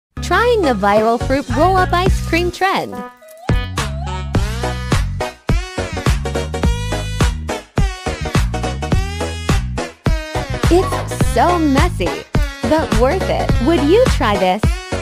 Trying the VIRAL Fruit Roll-up & ICE CREAM trend 😋 🍦🍒
Today I tried the viral Fruit Roll-up and ice cream trend! I didn't honestly believe this would actually work but oh man did it ever!!

Recipe:
Ingredients:
Fruit Roll-Ups

Preparation:
Unroll a Fruit Roll-up
Place a scoop of ice cream in the center
Fold the fruit Roll-up so the ice cream is contained inside
Wait about 30 seconds for the Fruit Roll-up to become solid